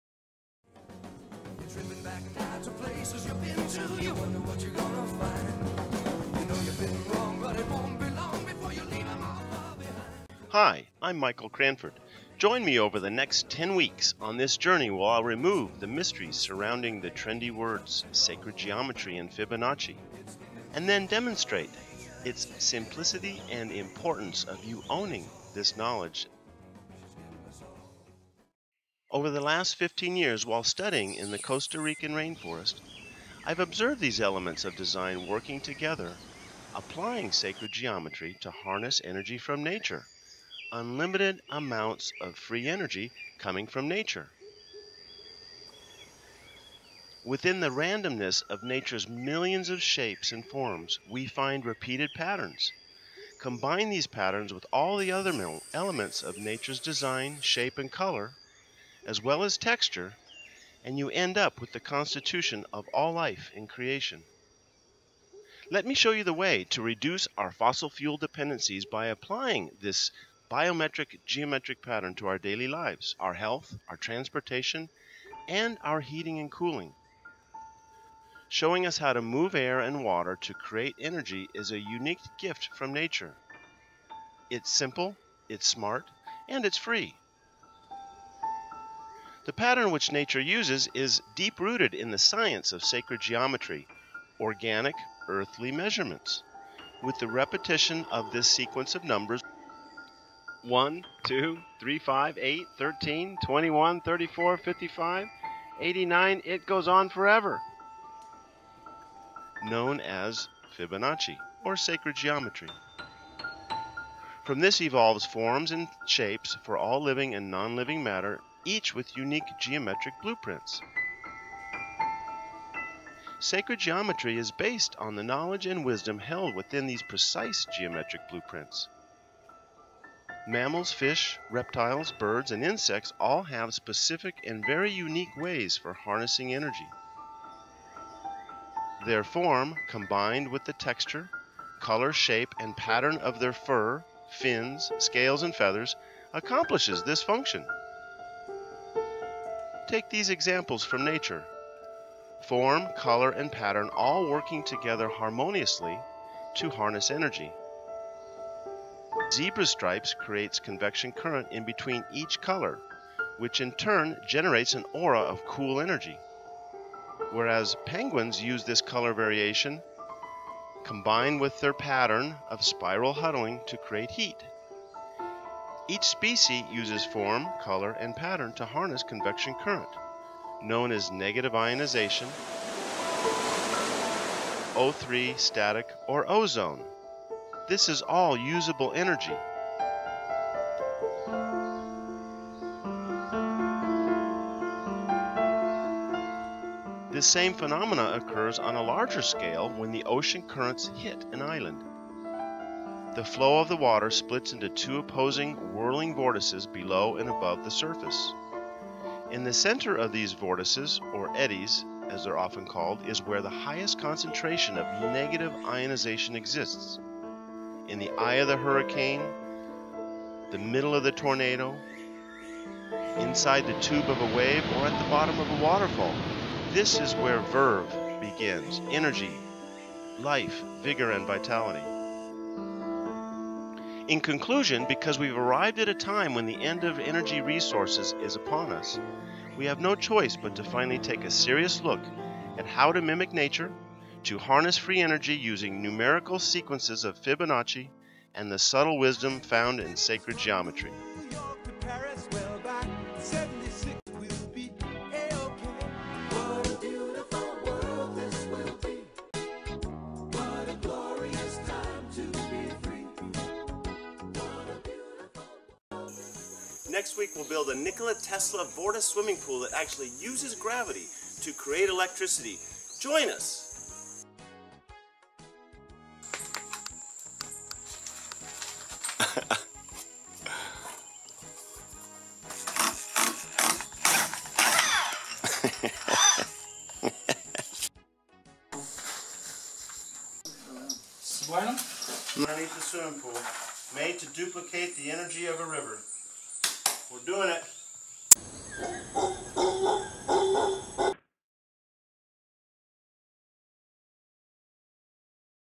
Sacred Geometry Architect Designing Homes to Creates Free Energy
Join me over the next 10 weeks to discover Sacred Geometry. l will remove the mystery surrounding the ‘trendy’ words Sacred Geometry and Fibonacci and then I’ll demonstrate the simplicity and the importance of you owning this knowledge as we approach the challenging times ahead.

Over the last 15 years while studying the Costa Rican rain forest, I’ve observed these elements of design working together applying sacred geometry to harness energy from nature—unlimited amounts of free energy.

Within the randomness of nature’s millions of shapes and forms, we find repeated patterns. Combine these with the other elements of nature’s design—shape, color and texture; you end up with the constitution of all life and creation.

Let me show you the way to reduce our fossil fuel dependencies by applying Sacred Geometry to our day-to-day lives—our health, our transportation and our heating and cooling. Showing us how to move air and water to create energy is a unique gift from nature—it’s simple, it’s smart and it’s free.

The pattern which nature uses is deep-rooted in the science of Sacred Geometry
(organic earthly measurements) with the repetition of this sequence of numbers (1, 2, 3, 5, 8, 13, 21 etc...) known as Fibonacci. From this evolves forms and shapes for all living and non-living matter each with unique geometric blueprints.

Sacred geometry is based on the knowledge and wisdom held within these precise geometric blueprints. Mammals, fish, reptiles, birds and insects all have specific and unique ways for harnessing energy. Their form, combined with the texture, color shape and pattern of their fur, fins, scales and feathers, accomplishes this function.

Take these examples of form, color and pattern working together harmoniously to harness energy. A zebra’s stripes create convection current in between each color, which in turn generates an aura of cool energy. Whereas penguins use this color variation combined with their pattern of spiral huddling to create heat. Each specie uses form, color and pattern to harness convection current (known as negative ionization), into usable energy in the form of static, O3 or ozone.

Michael H. Cranford